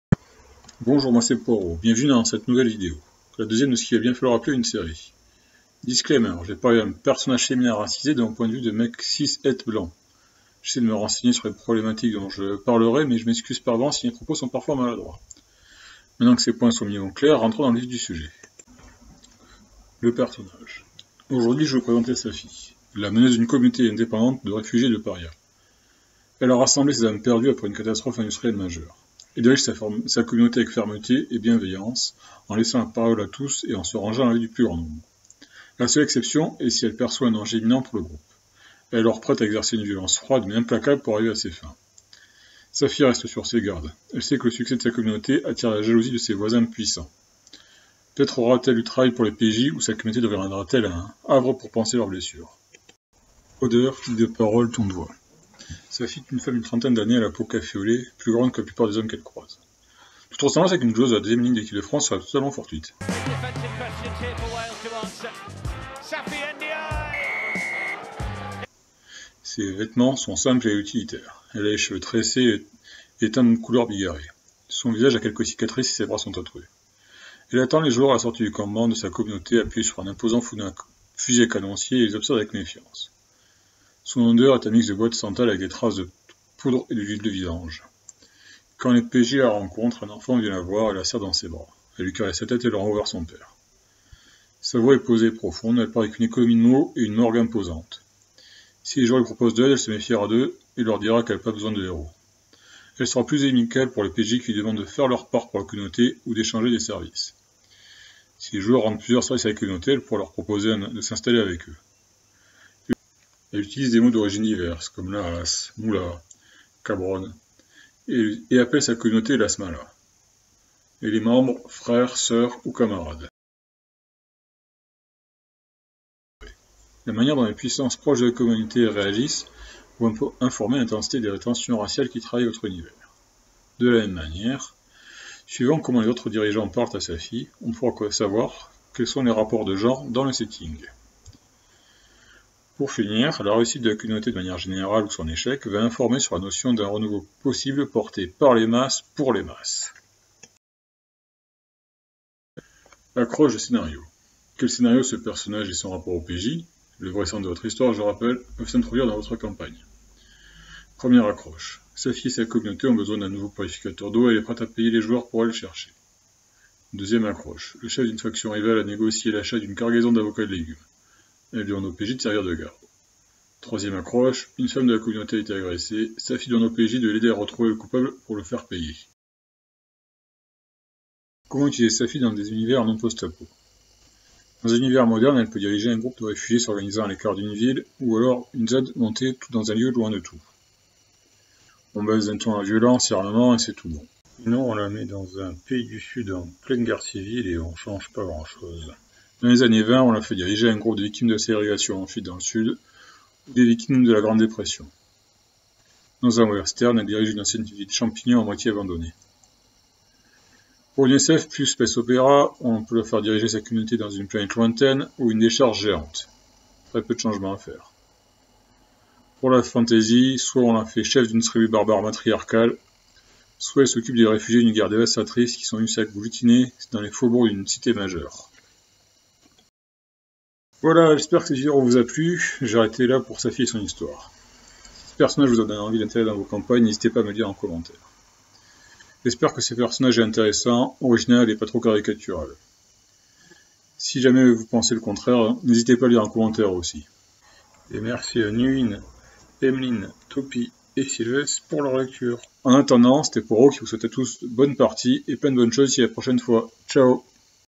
Safi Cheffe de communauté post apo
Cette vidéo vous présente Safi et sa communauté et la manière de l'intégré à vos partie de jeux de rôle.